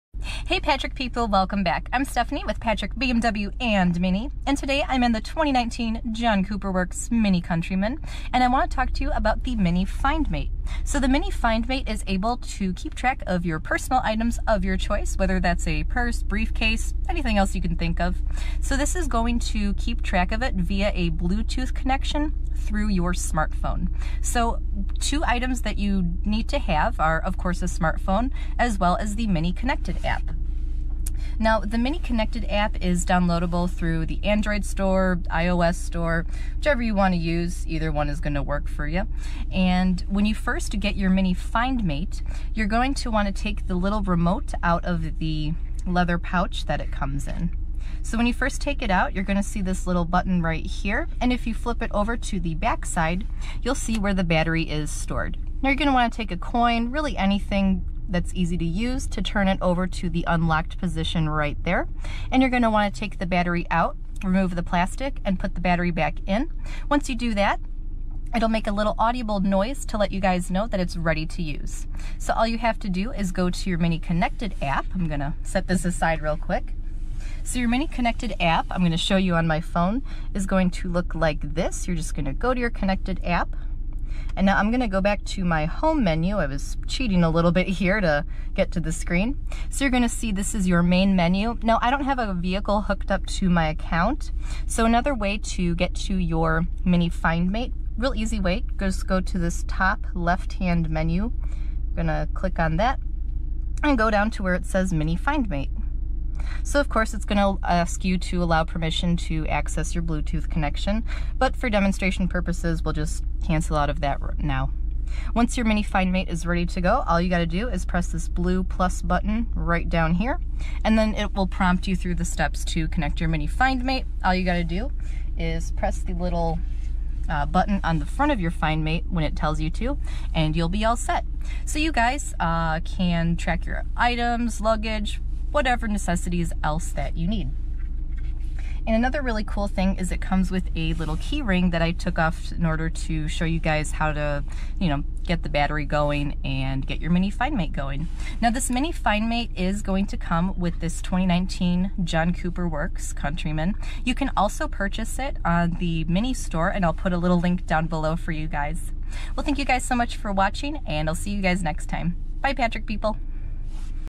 What is a MINI FindMate | Schaumburg IL | Patrick MINI
Hello Patrick People!

 I'd like to introduce the Mini FindMate! Keeping track of your personal items is a breeze thanks to bluetooth connectivity via your smartphone.

-Stephanie Wetteland
 BMW Genius
 Patrick BMW
 700 East Golf Road
 Schaumburg, IL 60173